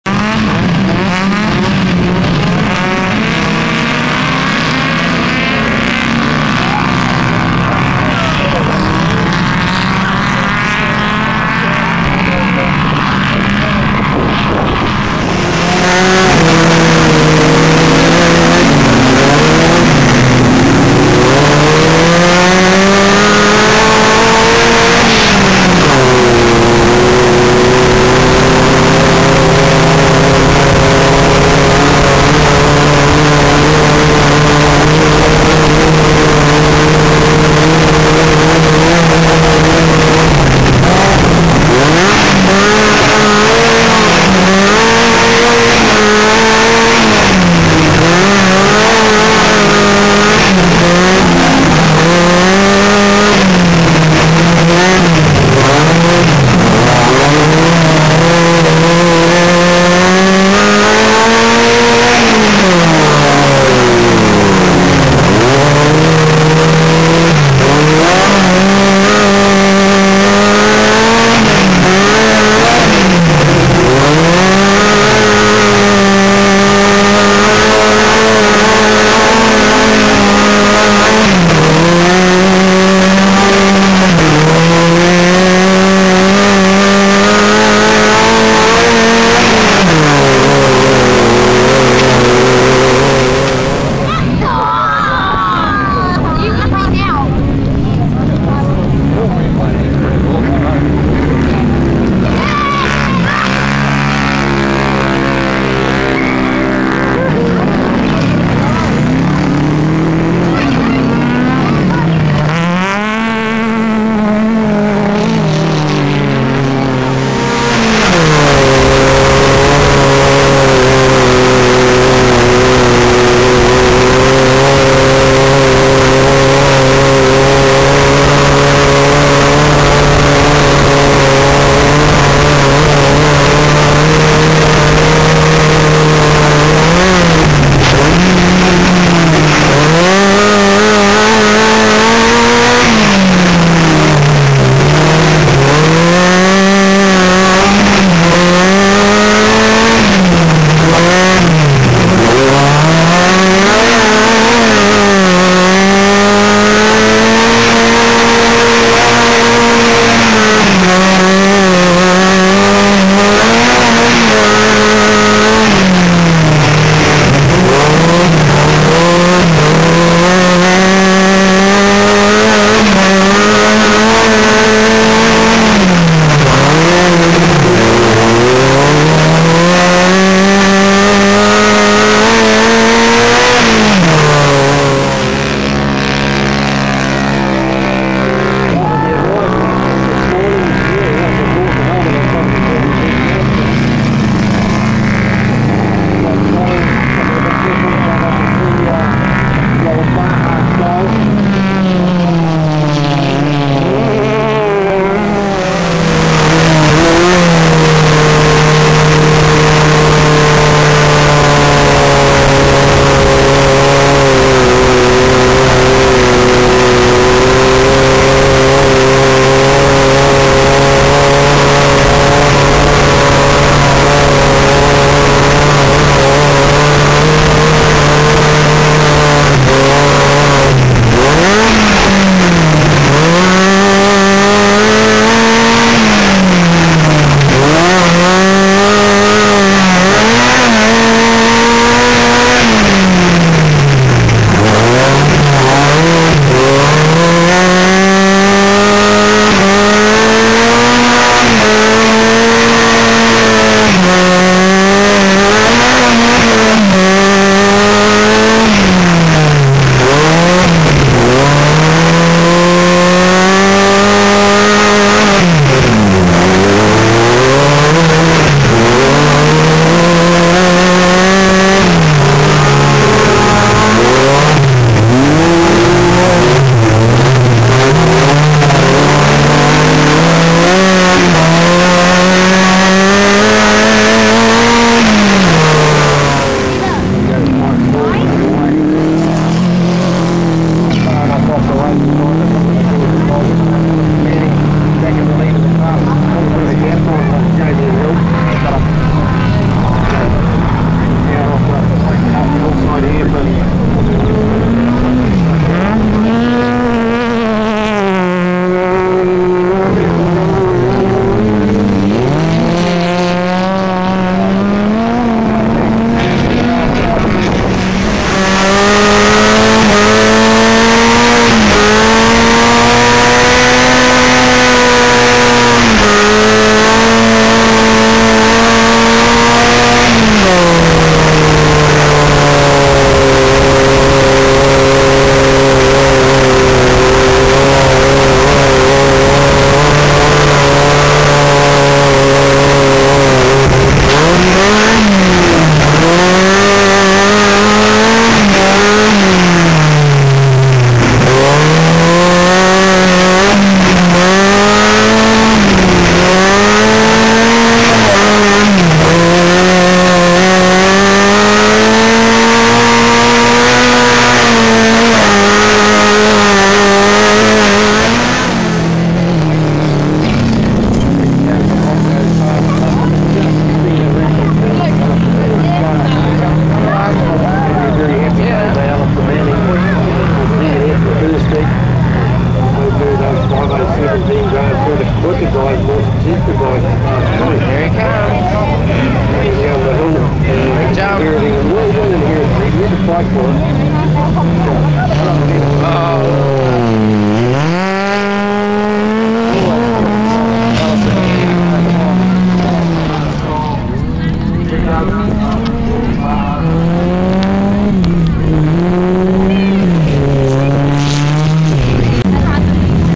Footage from Mangawhai round 2 nationals 2012
Edited footage onboard and spectator view from race 1 at the 2012 round 2 nationals at Mangawhai class 5